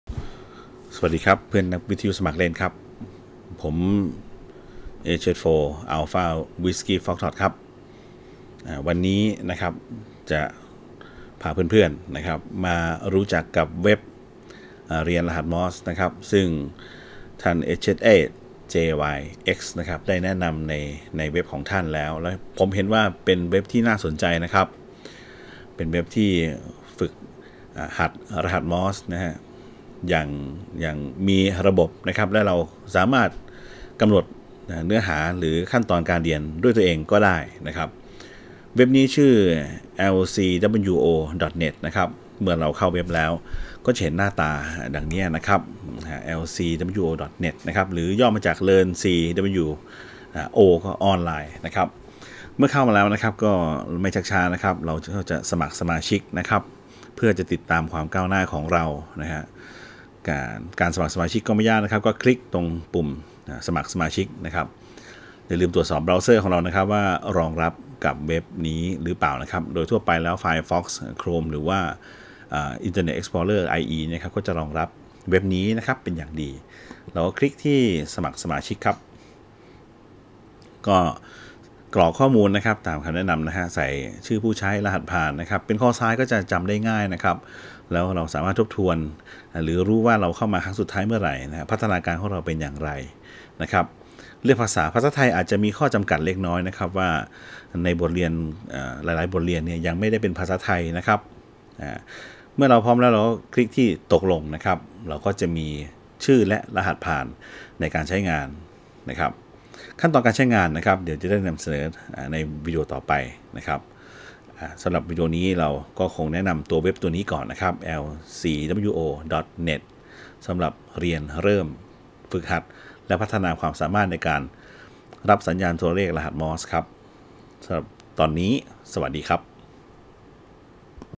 LCWO_01
เรียนรหัสมอร์สแบบ Koch ที่ lcwo.net ตอนที่ 1